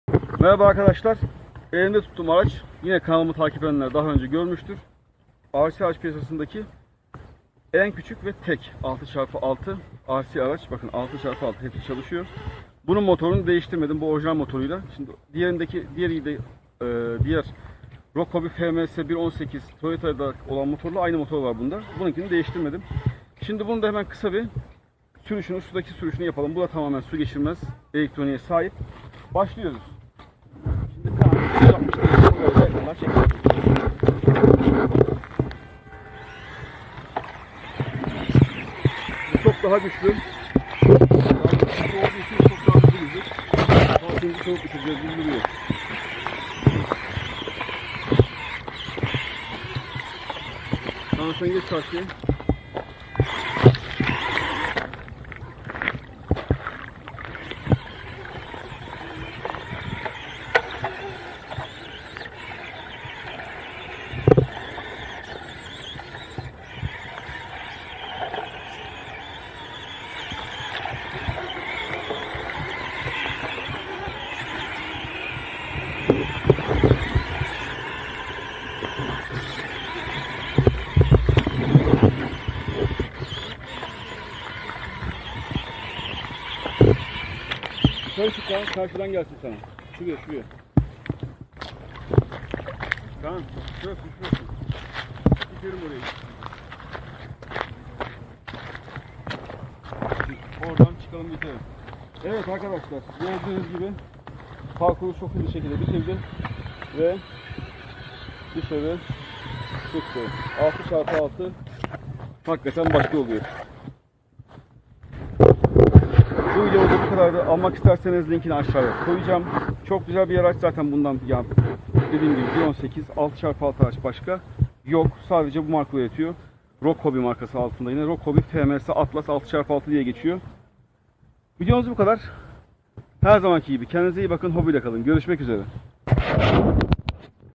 Dünyanın TEK 1/18 Ölçekli 6x6 RC Arabası - FMS ATLAS 6x6
FMS Atlas 6X6 1/18 Ölçekli RC Arazi Aracı Off Road performansı